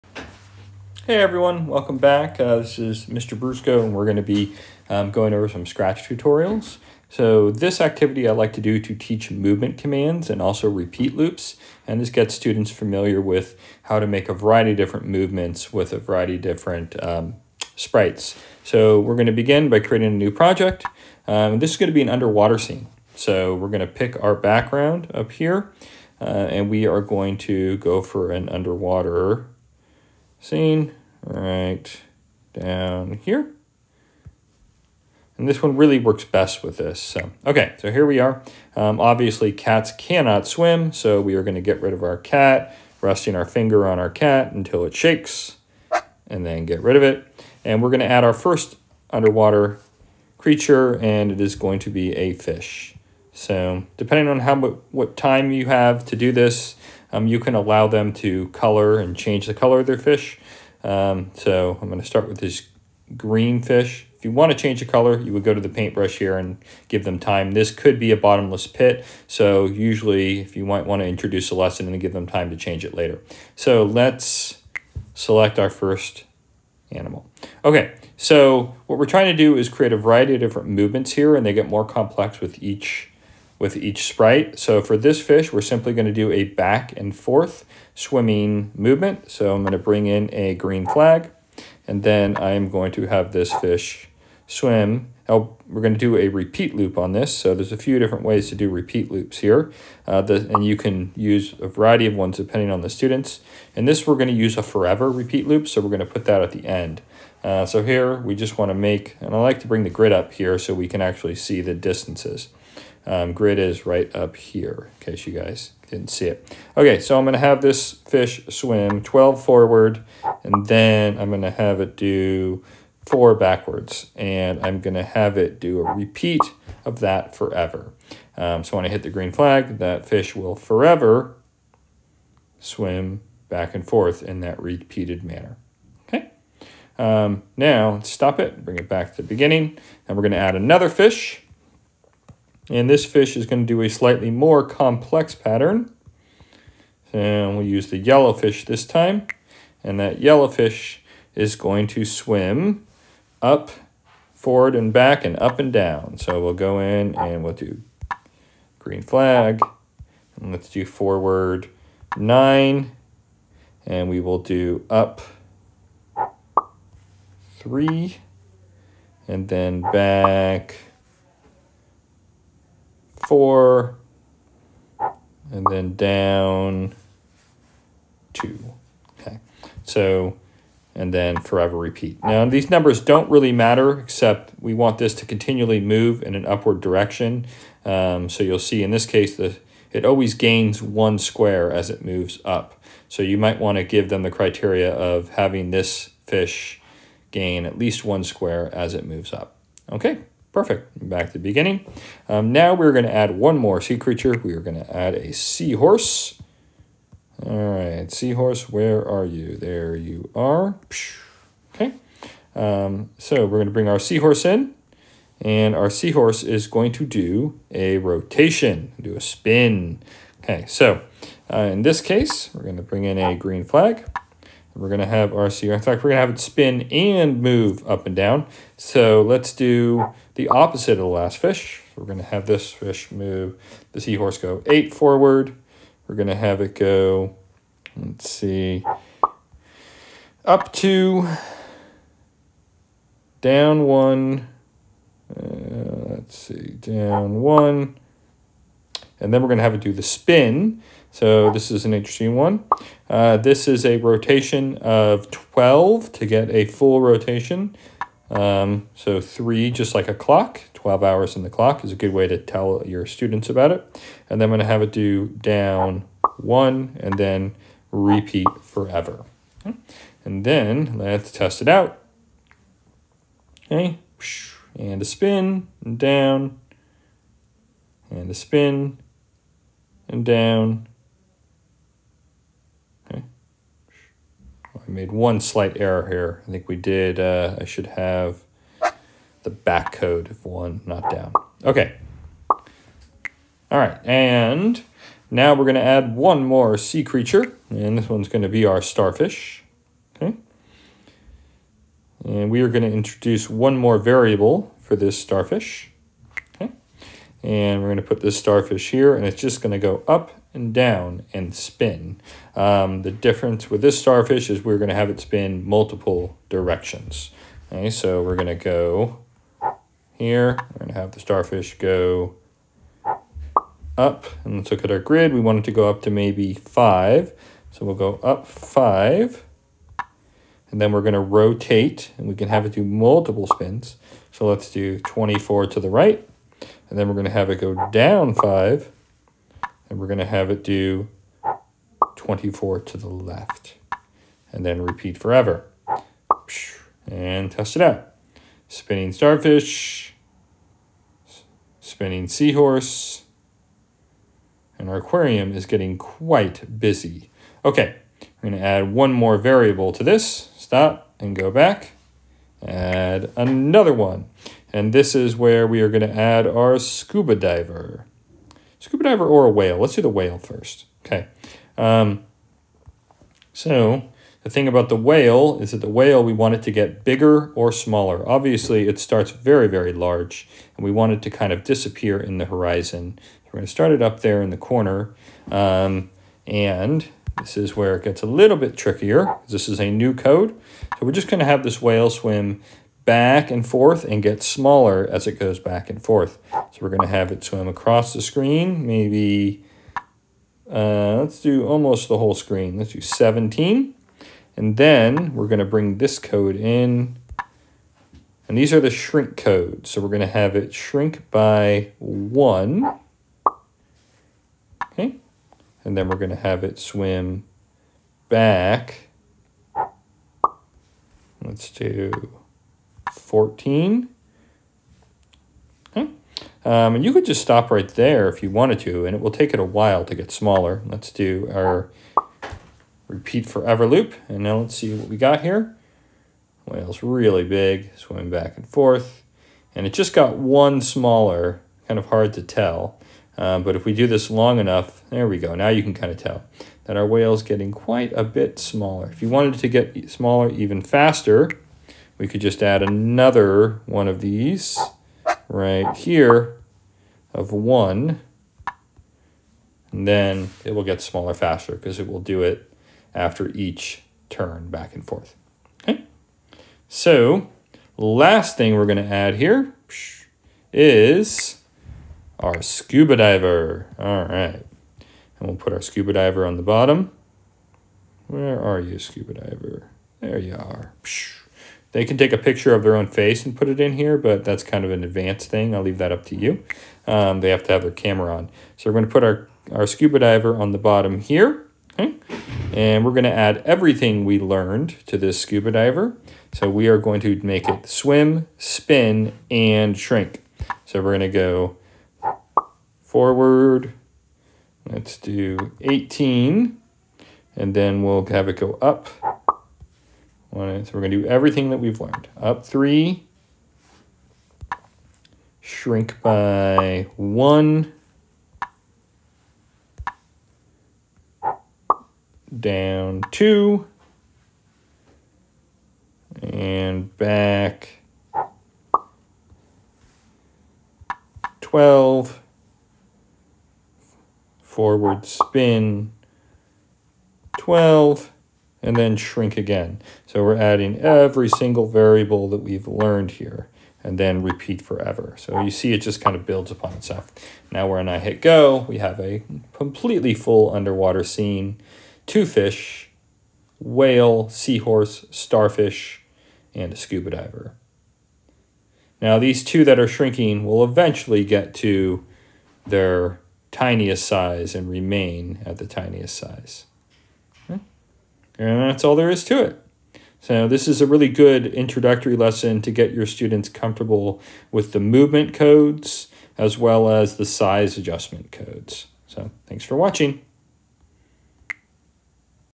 Scratch Jr. Make an Aquarium
Tutorial on building an aquarium in Scratch Jr. that helps teach a variety of movement, size, and speed codes.